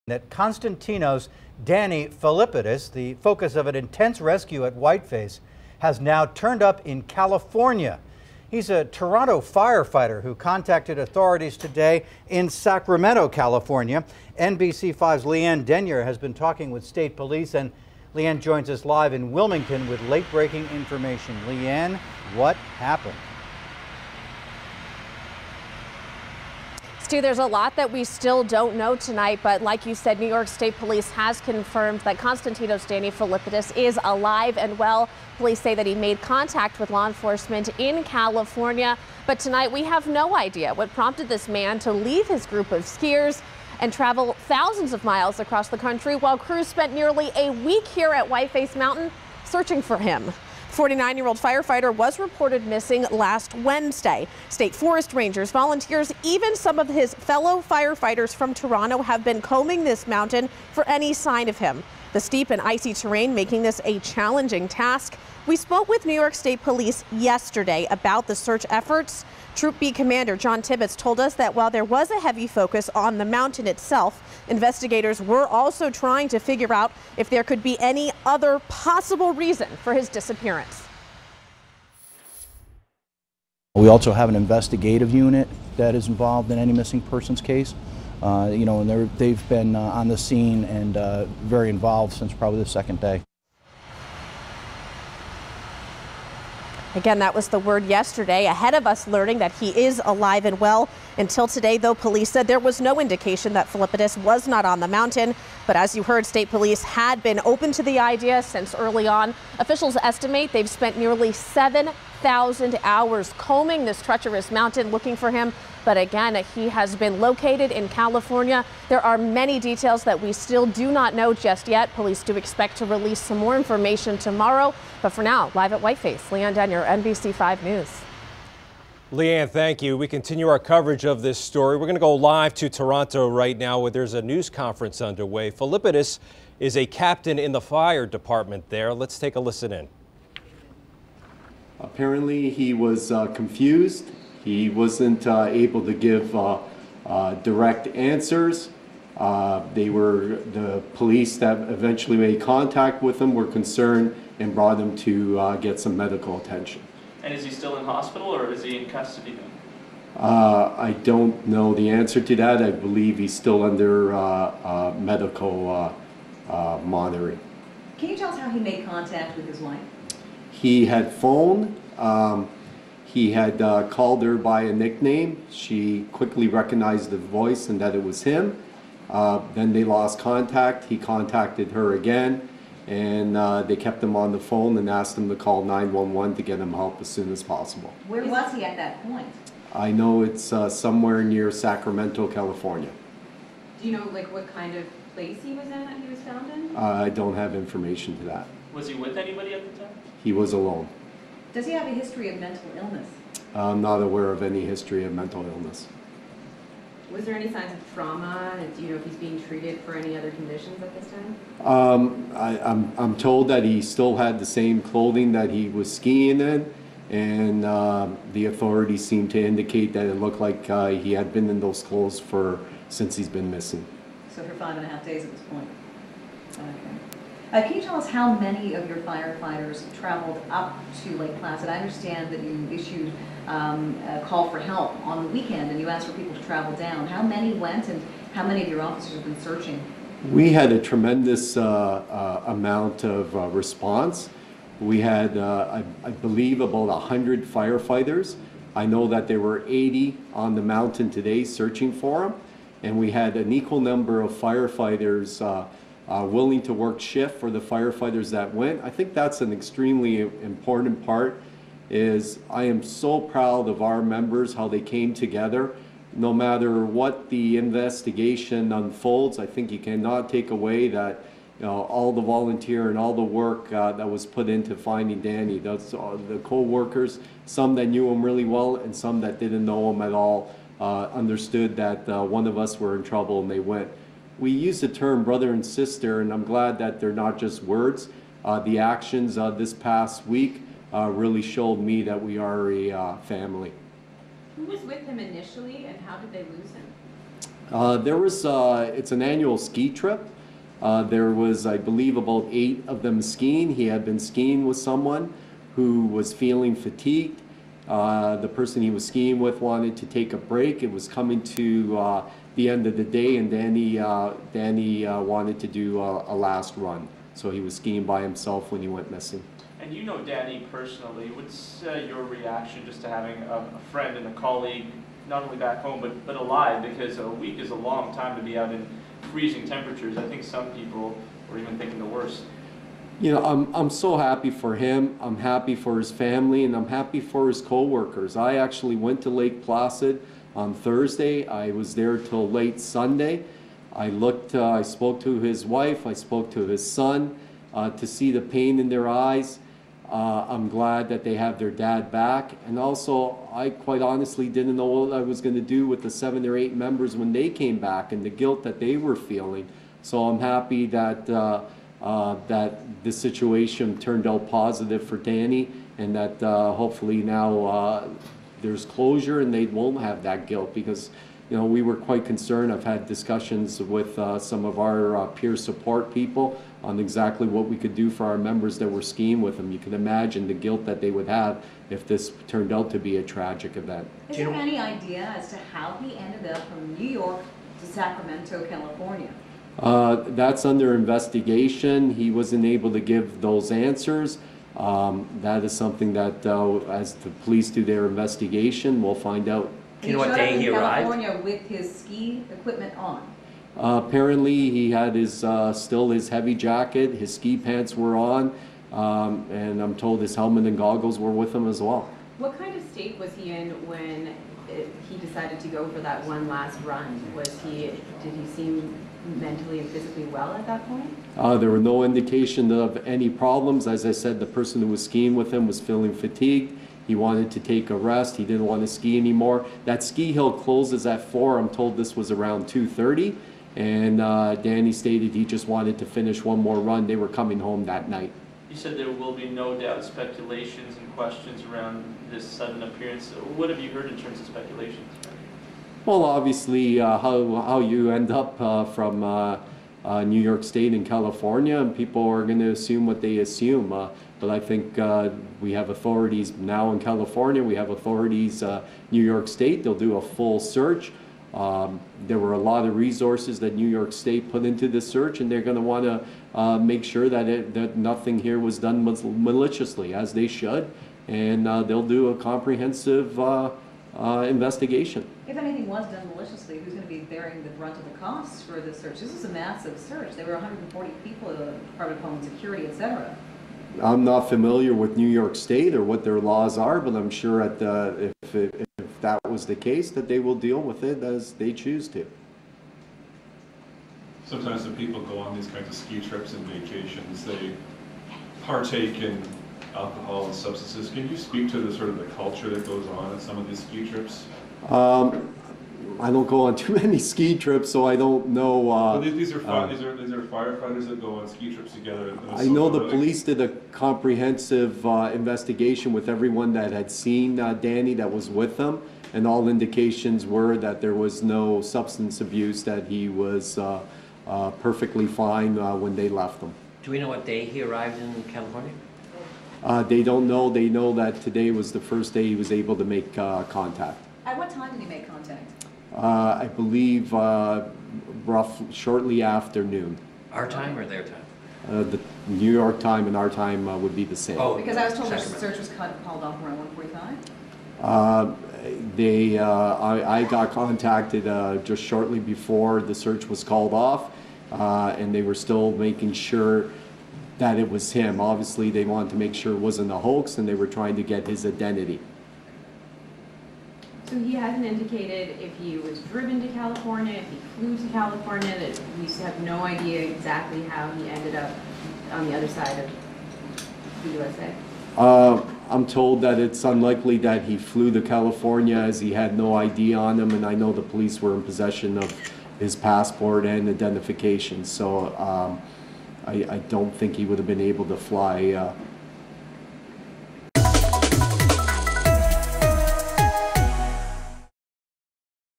Missing Whiteface skier found alive in California
A skier missing from Whiteface Mountain has been found in California.

New York State Police said Constantinos “Danny” Filippidis, 49, of Toronto, was found 2,900 miles away Tuesday in Sacramento.